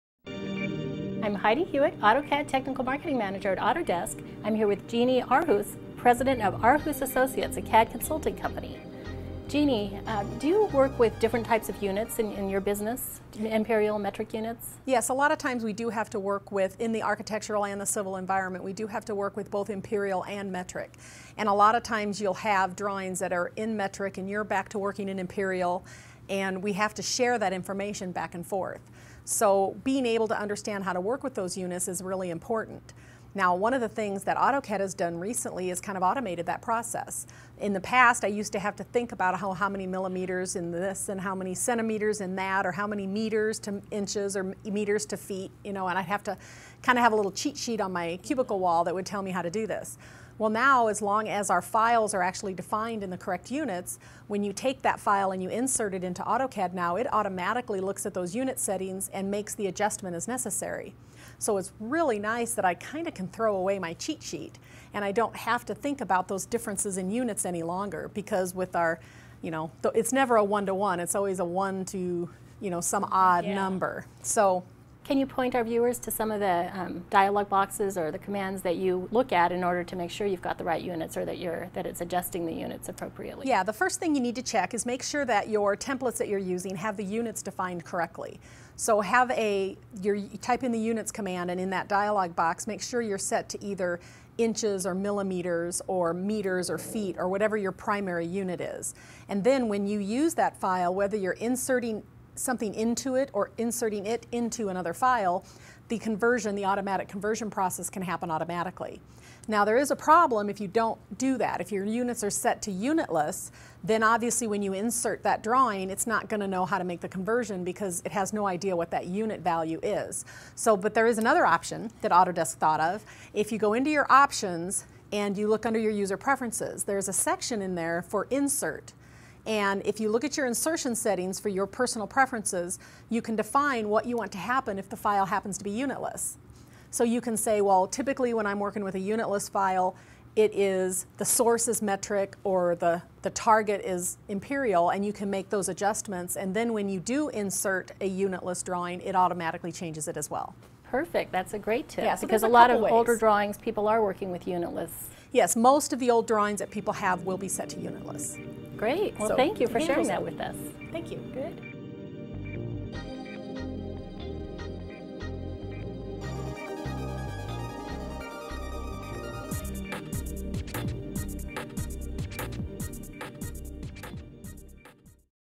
Jeanne Aarhus: Metric and Imperial Drawing Units
Heidi Hewett talks to Jeanne Aarhus, president of Aarhus Associates, about sharing files that are in metric or imperial units and how AutoCAD can automatically convert one to another.